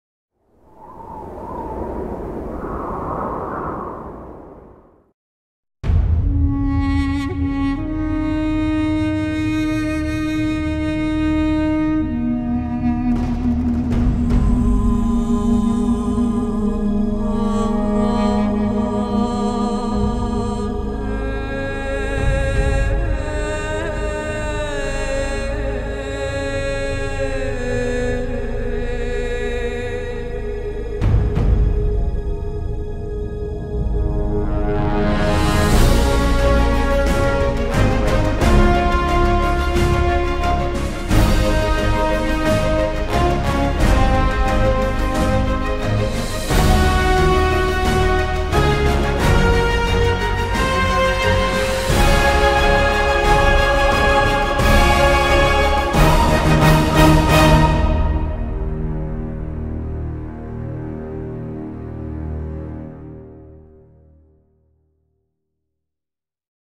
Svalbard Advert
A teaser clip from our Svalbard sailing expedition.